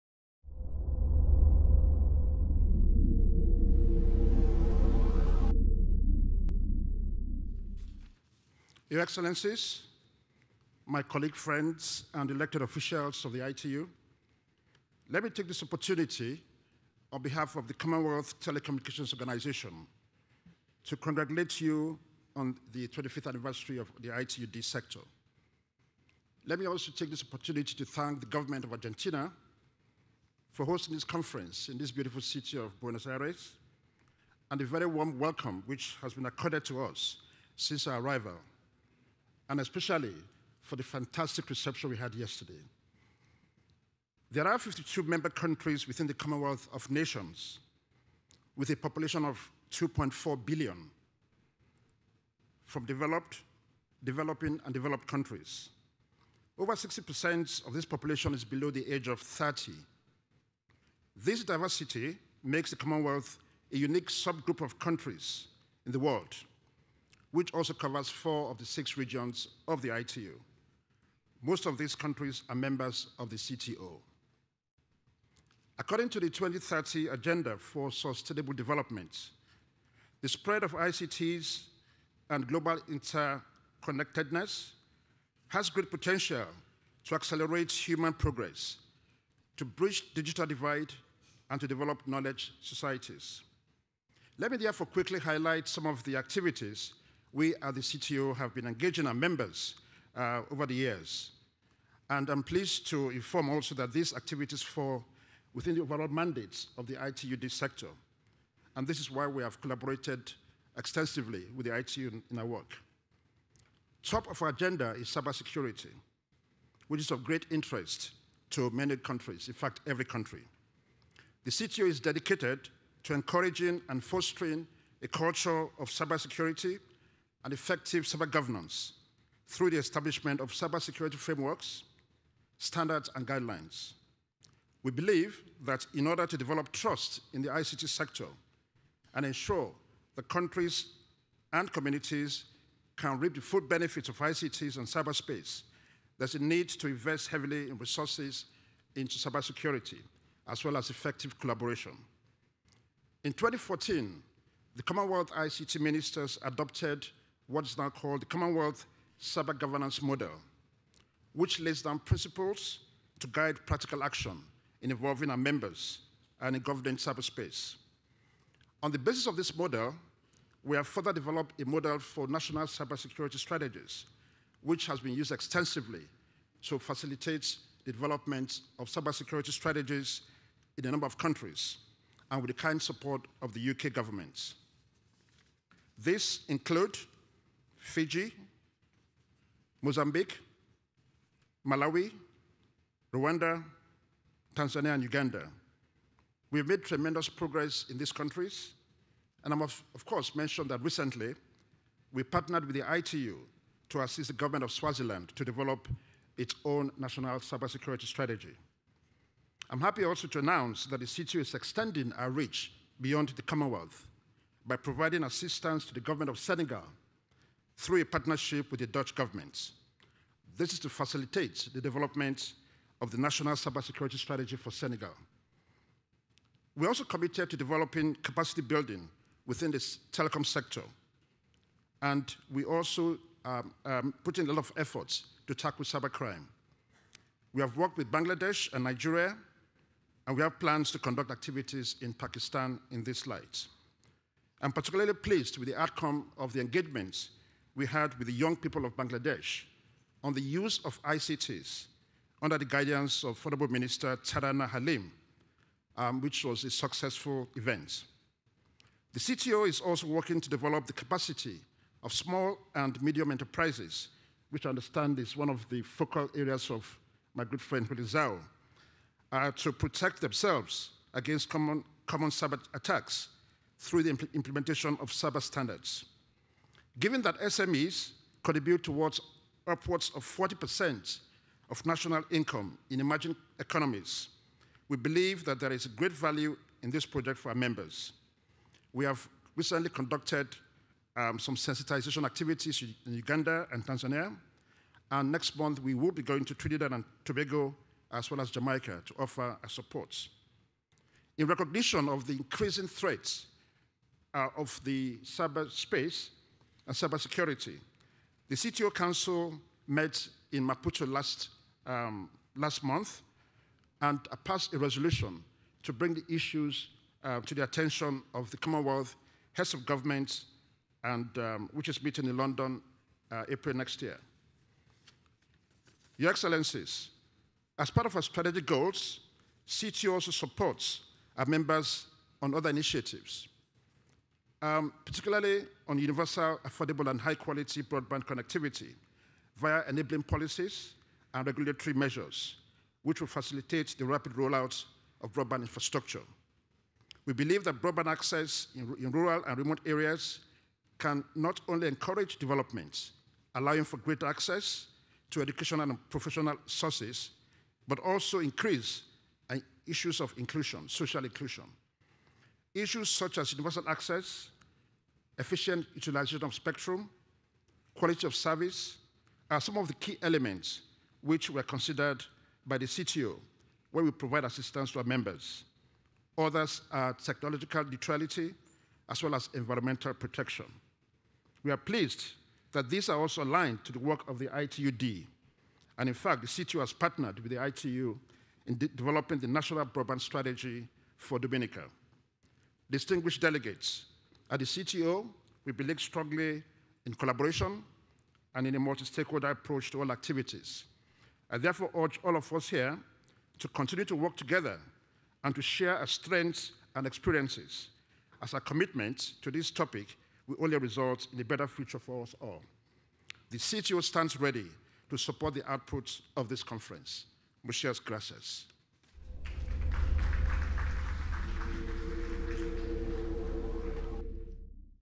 WTDC-17: Mr Shola Taylor, Secretary General, Commonwealth Telecommunications Organisation
Policy Statement at WTDC-17 by Mr Shola Taylor, Secretary General, Commonwealth Telecommunications Organization